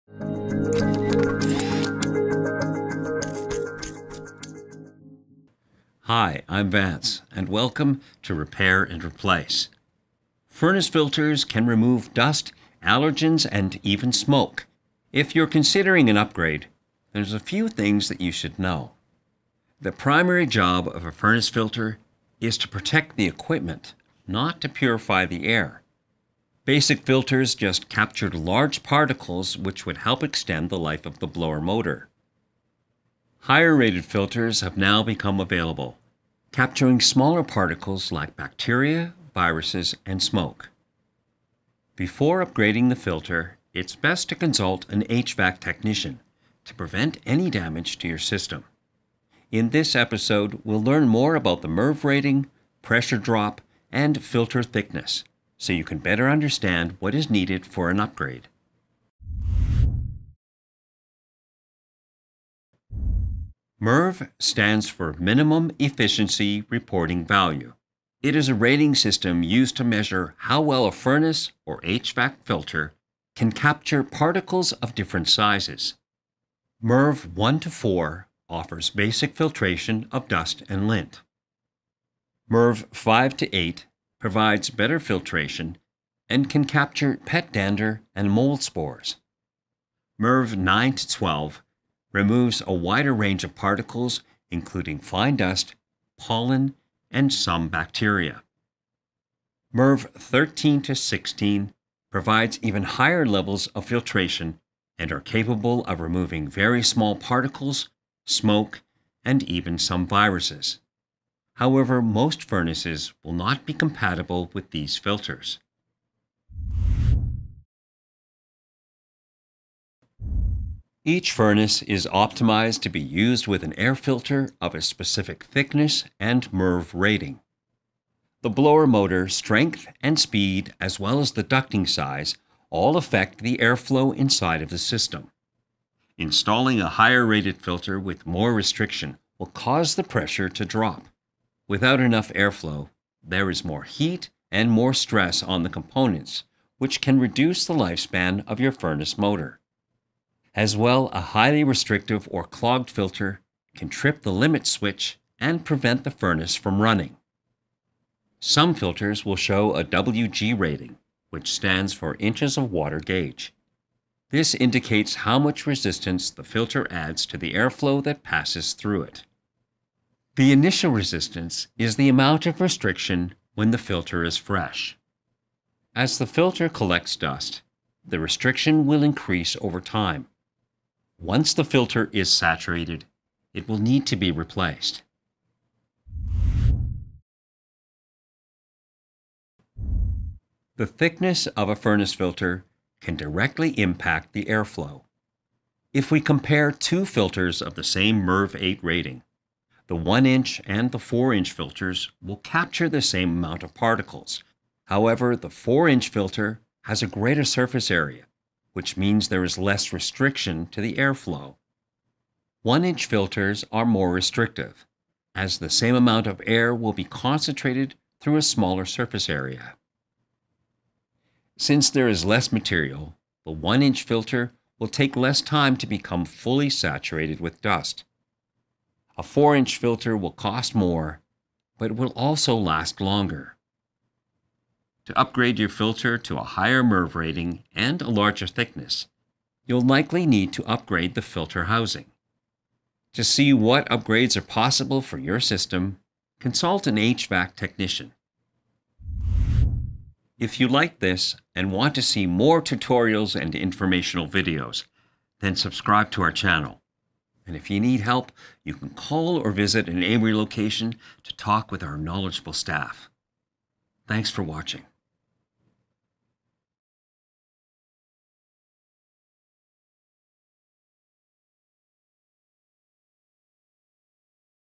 Furnace Filter Upgrade (What You Need to Know) | Repair & Replace
Can I put a higher rated filter in my furnace? Will a heavier filter restrict the airflow?
In this episode of Repair & Replace, Vance explains what you need to know before installing a better furnace filter. You'll learn about how MERV ratings, pressure drop, initial resistance and filter thickness affect what filters are compatible with your furnace. Many people want to install a higher rated furnace filter to get rid of smoke and allergens. Before upgrading your filter it's best to consult with an HVAC technician.